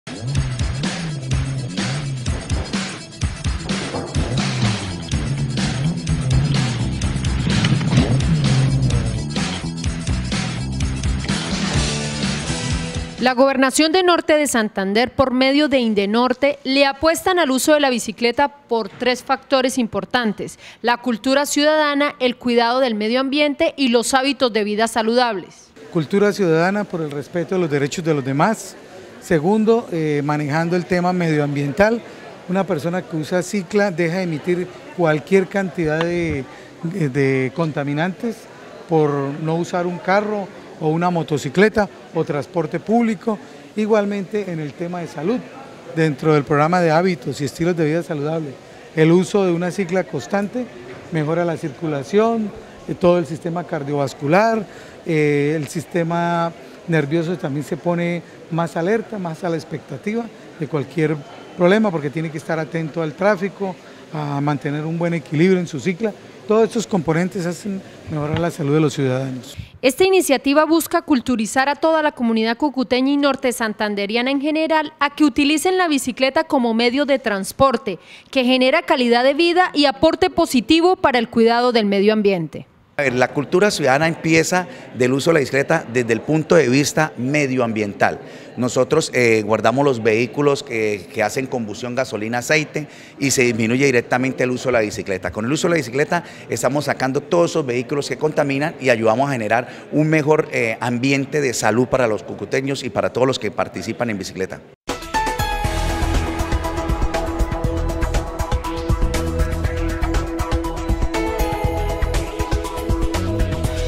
Primer Foro de la Bicicleta
EnVideo: conozca los detalles del Primer Foro Departamental de Bicicleta, que se efectuará el próximo viernes (15 de julio) a las 8:00 de la mañana en el centro comercial Ventura Plaza de Cúcuta.